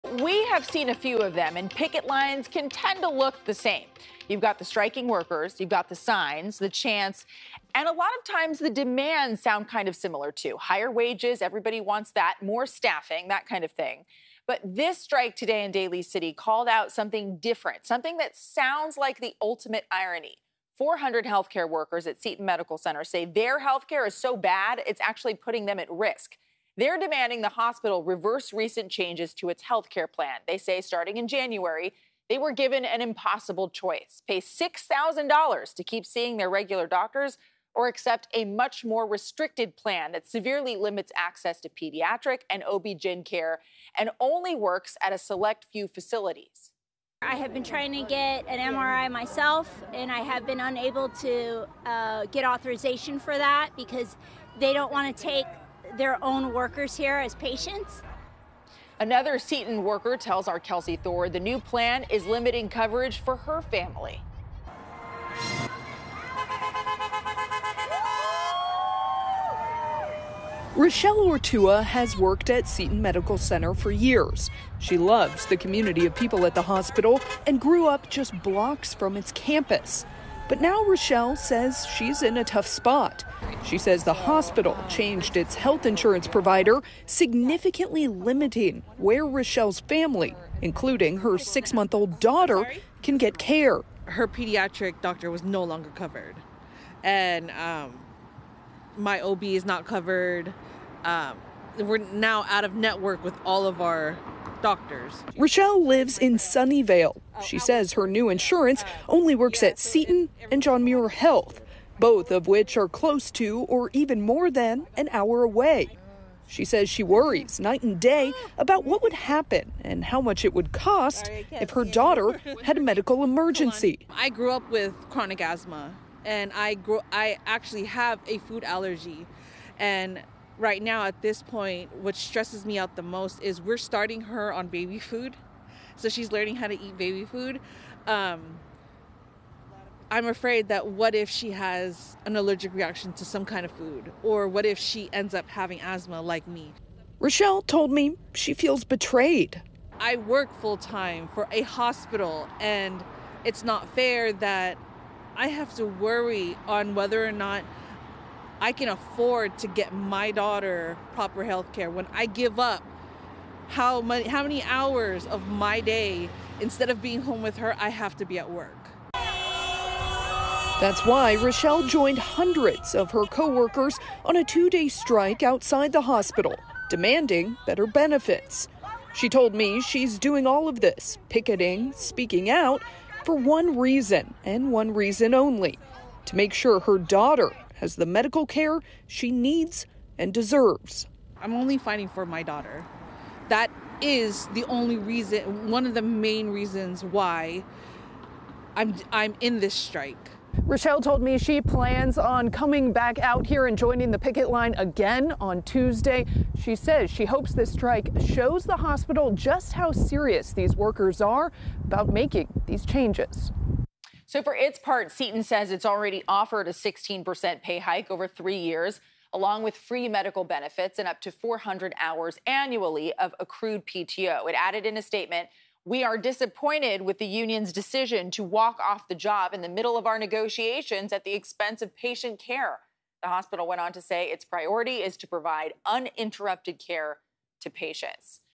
Health care workers strike at Seton Medical Center to demand better health care plan
Kelsi Thorud reports on a two-day strike at Seton Medical Center.